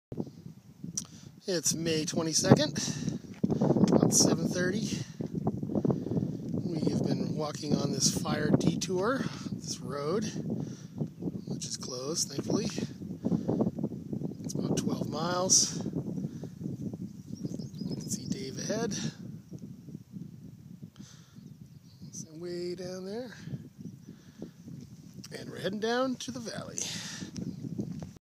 Morning roadwalk
Halfway through a long detour on a closed road